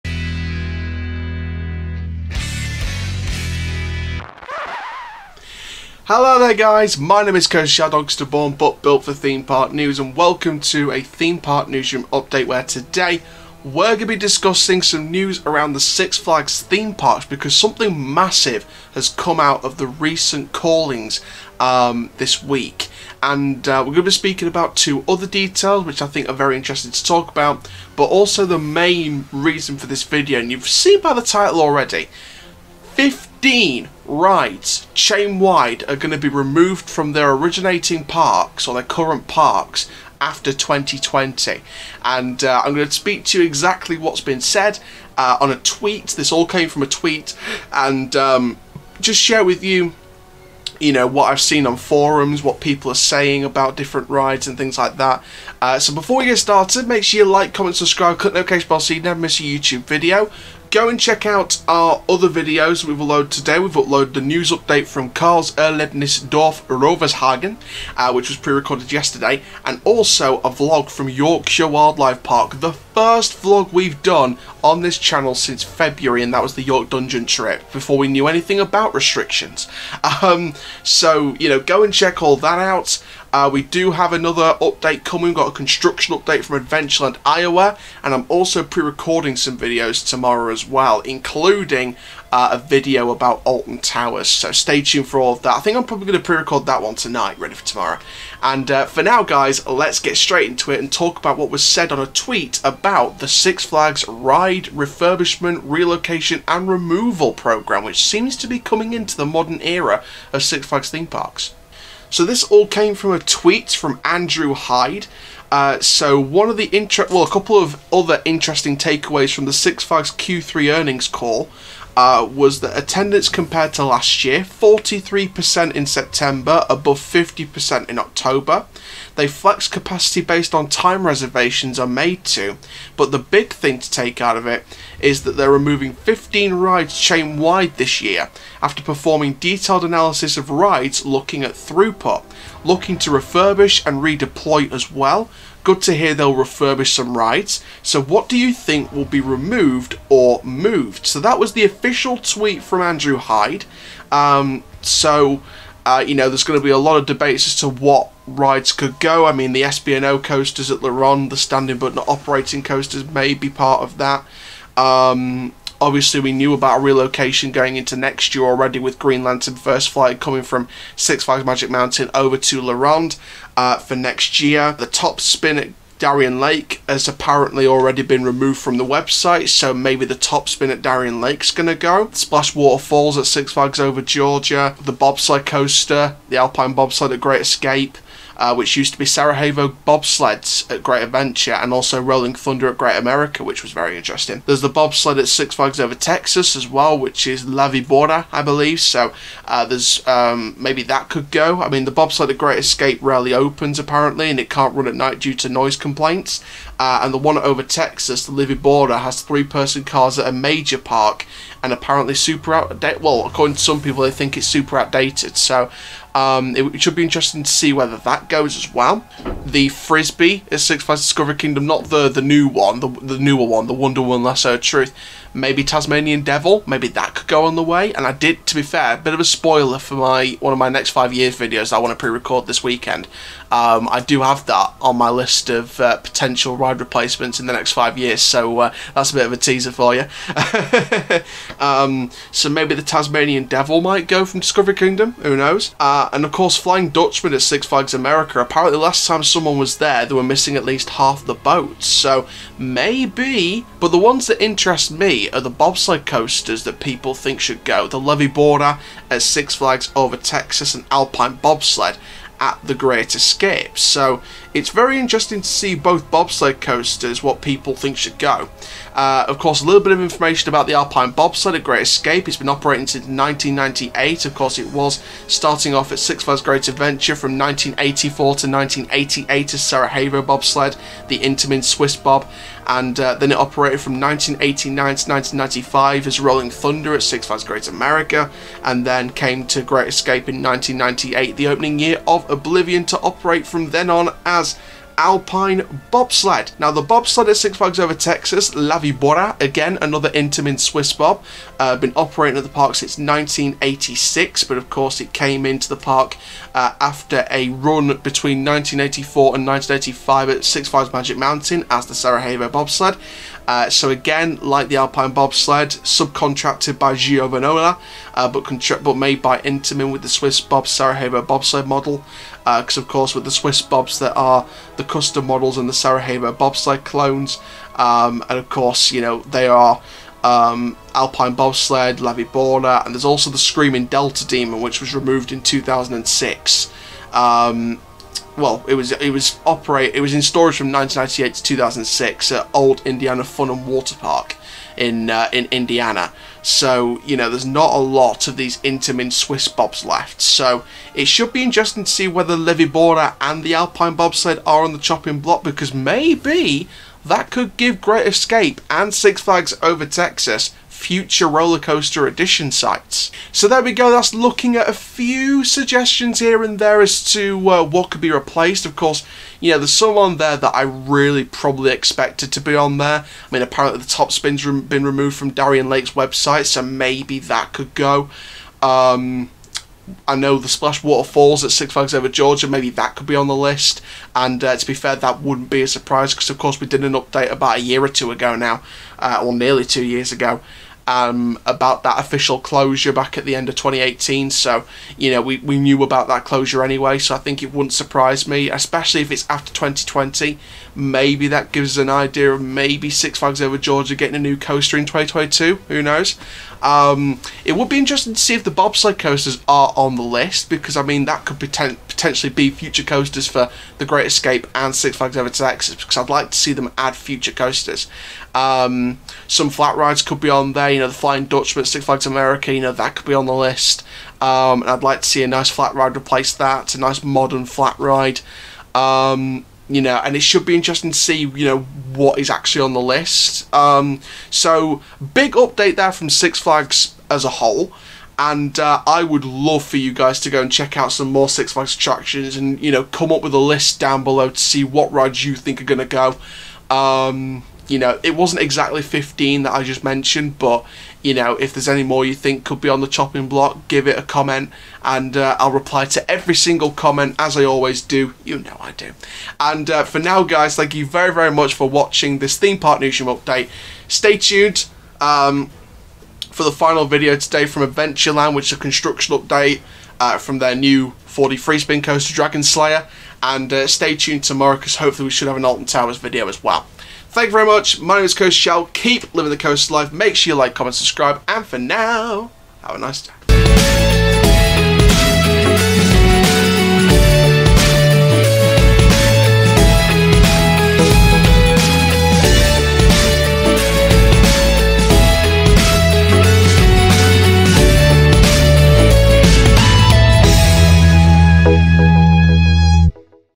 Six Flags Will Be Removing 15 Rides and Attractions Before the End Of the 2020 Season
On this episode of Theme Park Newsroom, I'll be sharing news that Six Flags will be removing or refurbishing and redeploying 15 rides and attractions chainwide after the 2020 season has finished. I'll share my thoughts on this news, alongside other details from the same source surrounding Six Flags, as well as predicting which rides and attractions could be on the chopping block.

Intro: 0:00
Facts and Stats: 1:52
My Thoughts: 2:34
Outro: 8:03